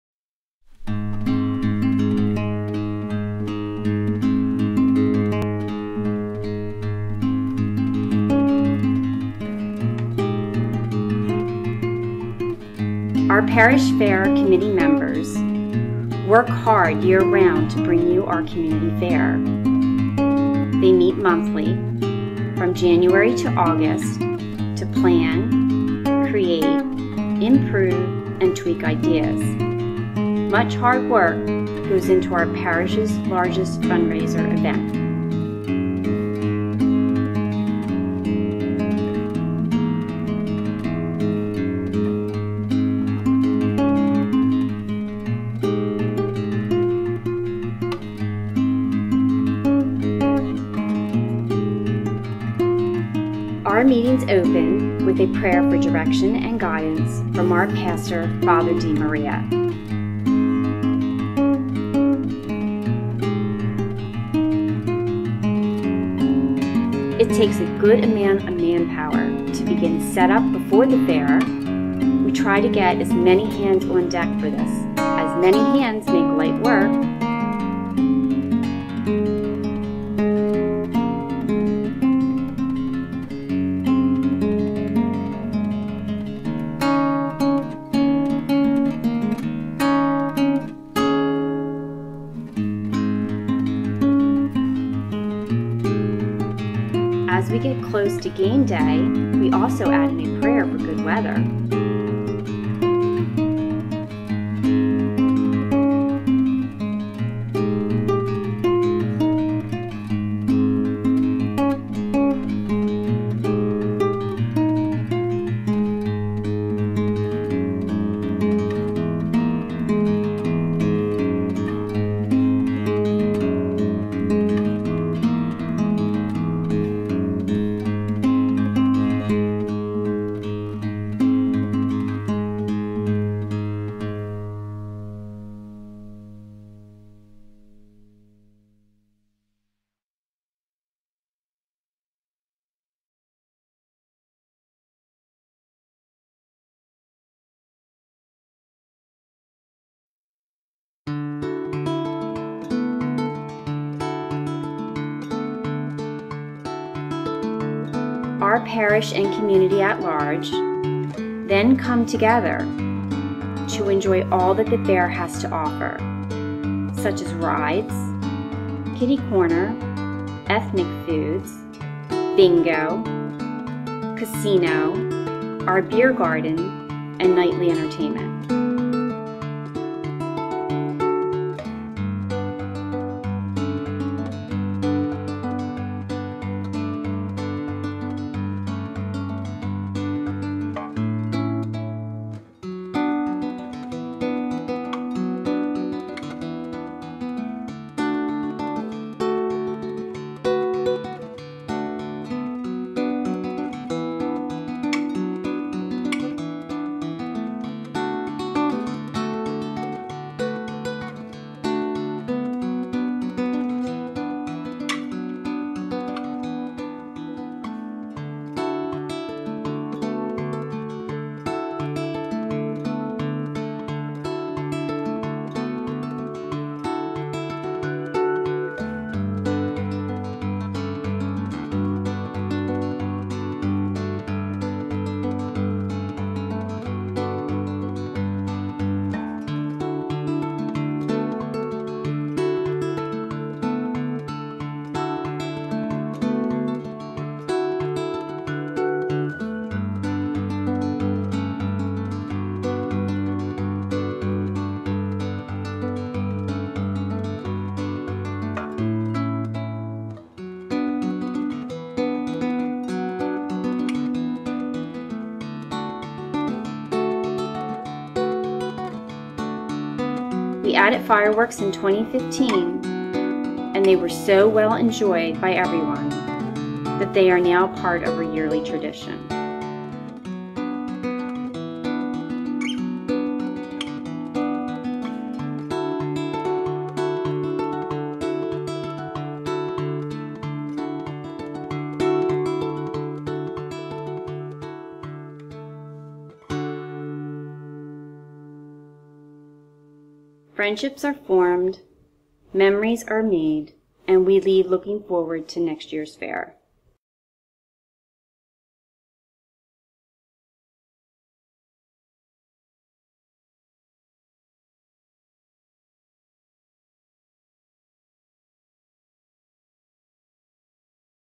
2016 Sacred Heart Fair
Recap of the 2016 Sacred Heart Fair in Royersford, PA.